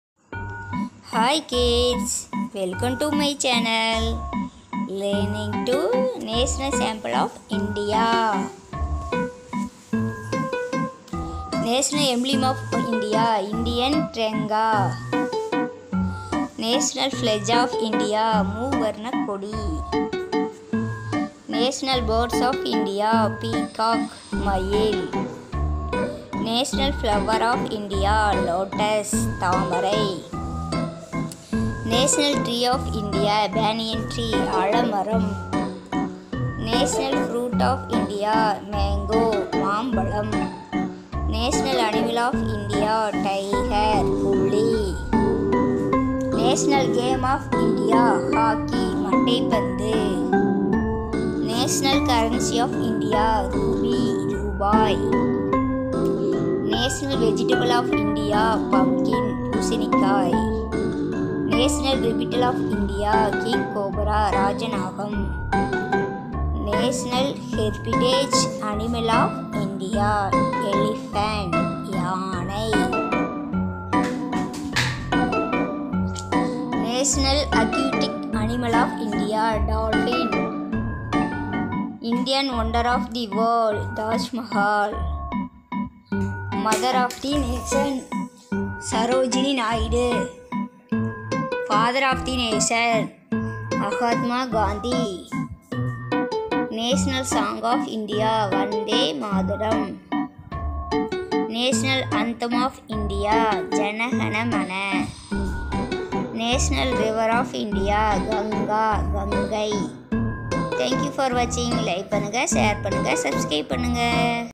National symbols of India_இந்திய தேசிய சின்னங்கள் தமிழ் & English language learning video for kids
National symbols of India_இந்திய தேசிய சின்னங்கள் தமிழ் மற்றும் ஆங்கிலம் learning video for kids